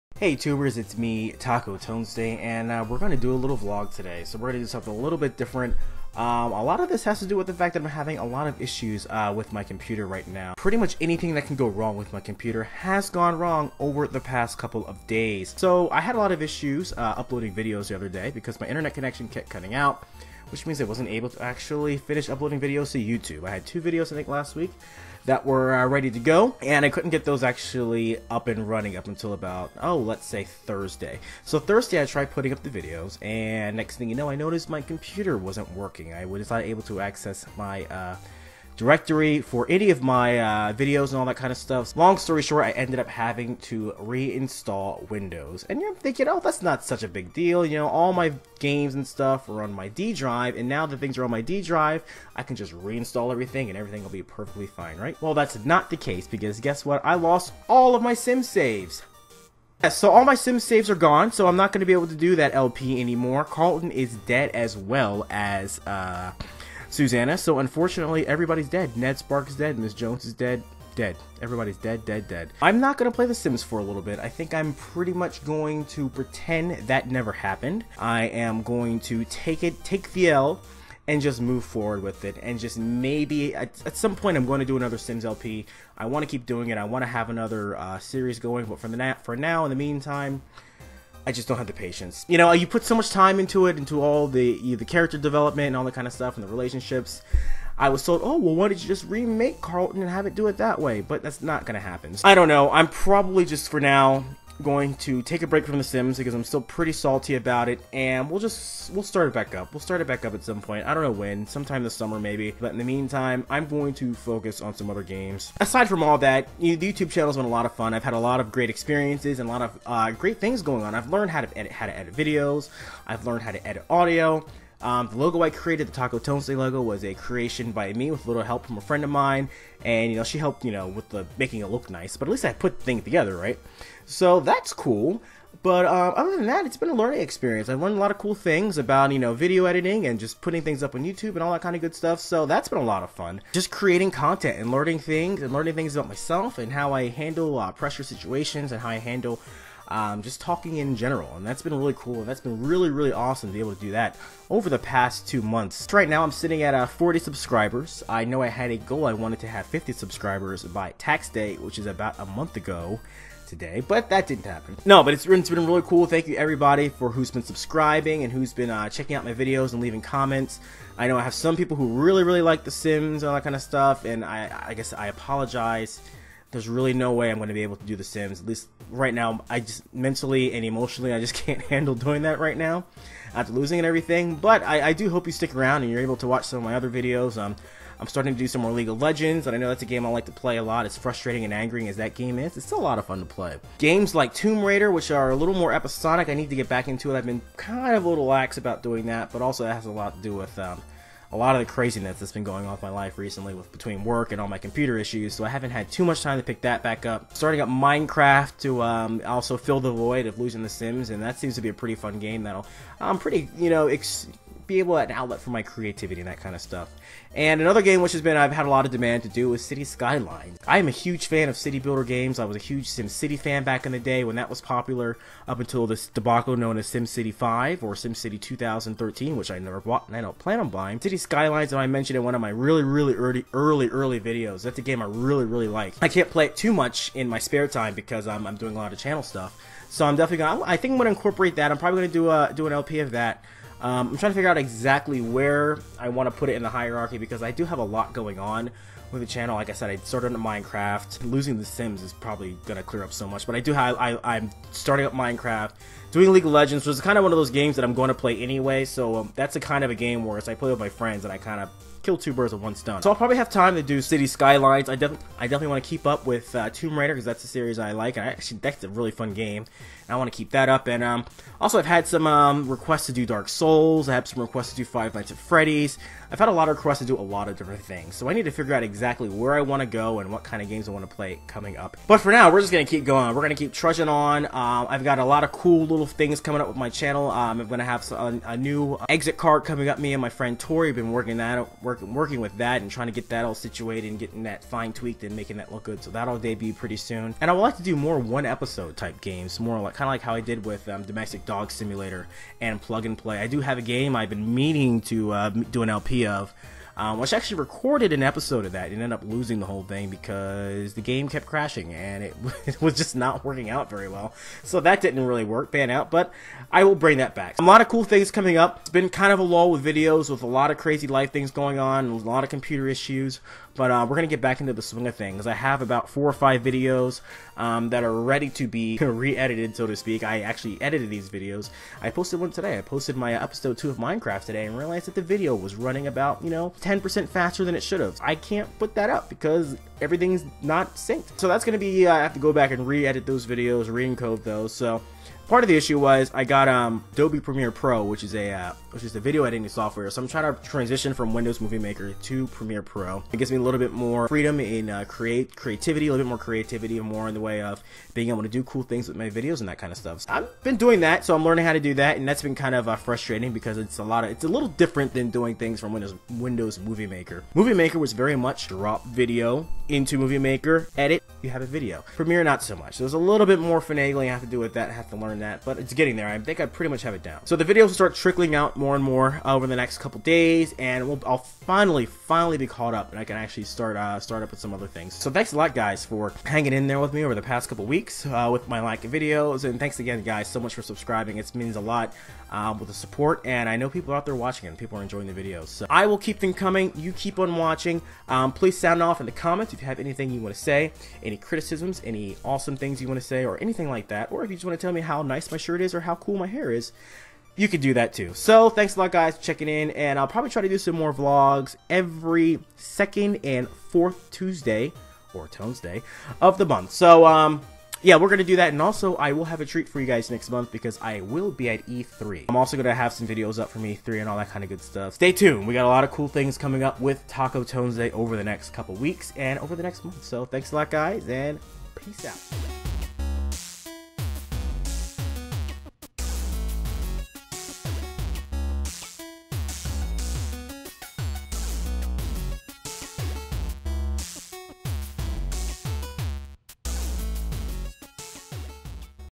Tonesday Vlog #1 - Channel Update/E3 Announcement
Today on the Tonesday Vlog I chat a bit about my channel, the recent struggle, and I have an announcement about E3!